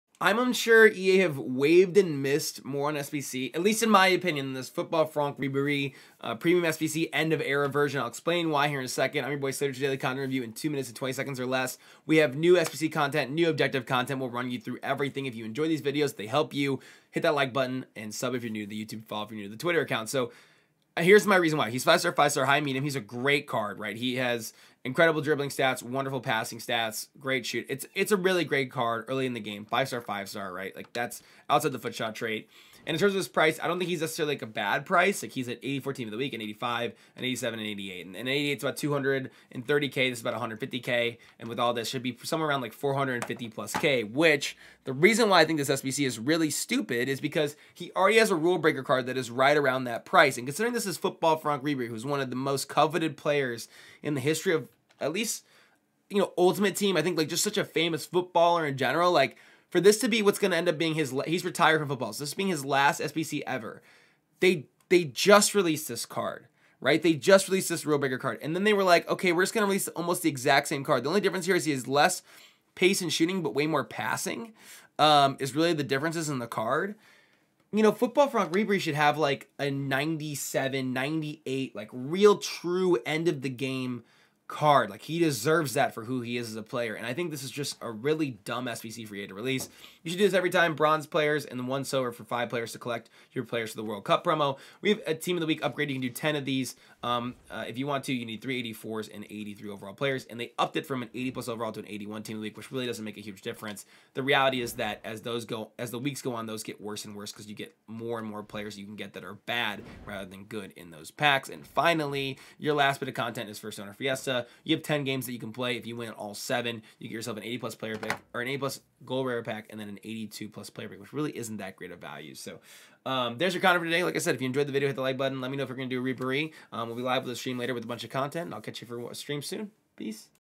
Why Ribery's End Of Era SBC Is A HUGE MISS From EA! FIFA 23 - Daily Content Review
Daily Content Review:
- End Of Era Ribery SBC
- First Owner Fiesta OBJ
- 81+ TOTW SBC
- 75+ World Cup Upgrade SBC